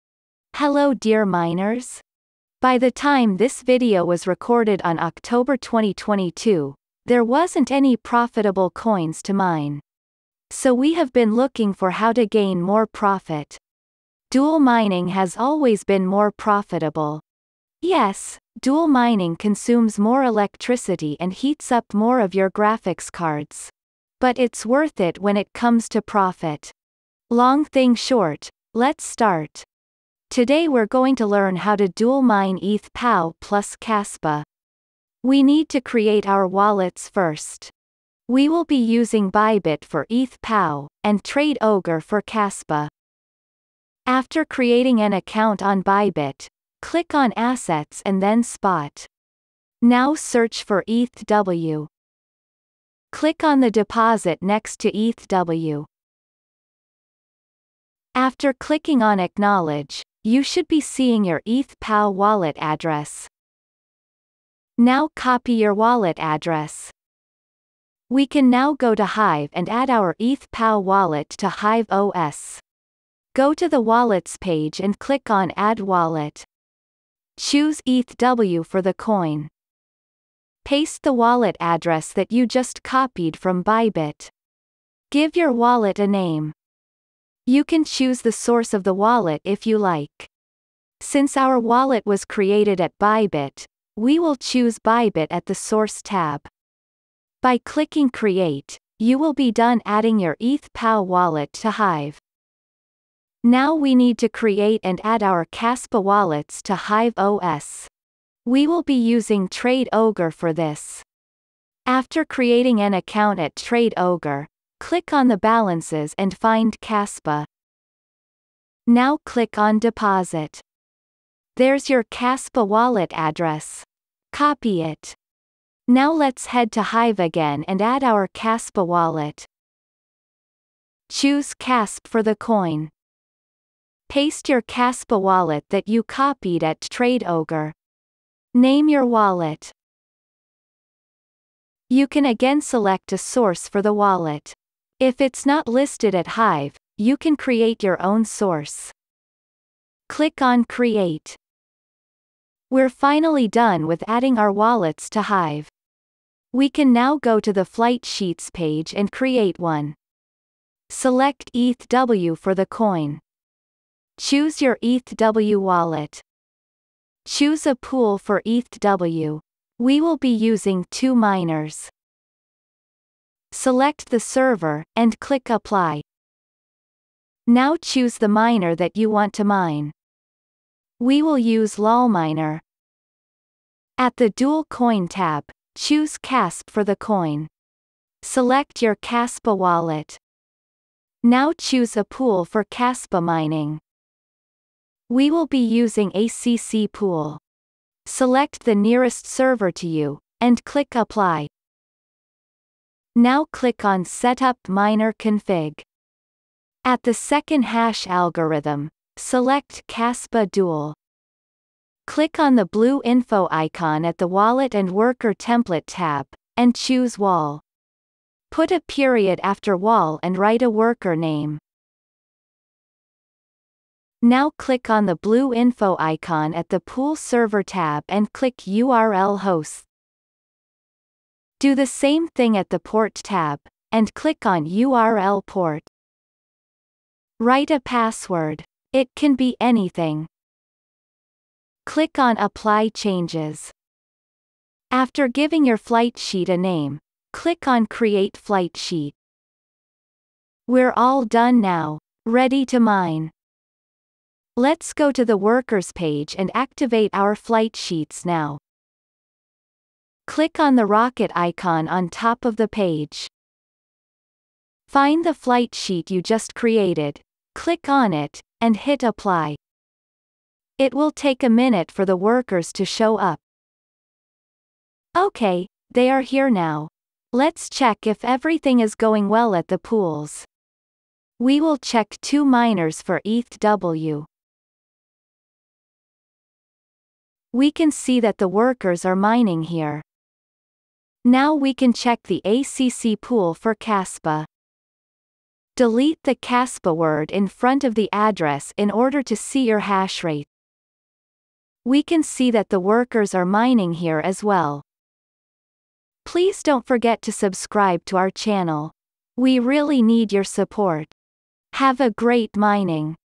Dual mining ETHW + KASPA | How to mine ETH POW + KASPA on HiveOS
How to dual mine ETH POW + KASPA using HiveOS
Today we will be learning how to create ETHW wallet, how to create KASPA wallet, how to create a flight sheet on HIVEOS and how to mine dual ETH POW + KASPA
Dual mining is more profitable than normal mining. So we decided to show you how to set a flight sheet and how to dual mining ETHW+KASPA.
We have used LolMiner, 2MINERS and ACC Pool.
You can also learn how to add wallet to HiveOS and how to create a flight sheet on HiveOS. ETHW wallet, KASPA wallet.
Enjoy your time watching the video while learning how dual mining works on ETHPOW+KASPA.
Have a great mining.

00:00 Dual mining ETHW + KASP
00:37 How to create ETHW wallet
01:12 How to add ETHW wallet to HiveOS
01:42 How to create KASPA wallet
02:09 How to add KASPA wallet to HiveOS
02:36 How to create flight sheet on HiveOS
04:36 How to check mining pools